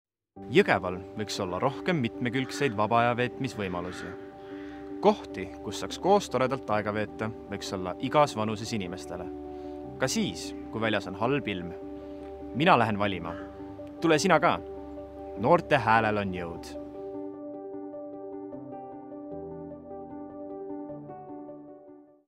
Noorte häälel on jõud / Joosep
Kohalikel valimistel saavad valida kõik vähemalt 16-aastased püsivalt Eestis elavad inimesed.

Esmavalija Joosep Jõgevalt räägib, miks tema kindlasti kohalikel valimistel hääletab.

KOV valimised toimuvad 17. oktoobril. Valimisnädal algab esmaspäeval, 11. oktoobril ja koosneb kuuest eelhääletamise päevast ning lõpeb pühapäeval, valimispäeval. Eelhääletamise perioodil on võimalik oma hääl anda ka elektrooniliselt.

Noorte häälel on jõud!

Video on valminud koostöös Riigi valimisteenistusega kohalike omavalitsuste volikogu valimisteks aastal 2021.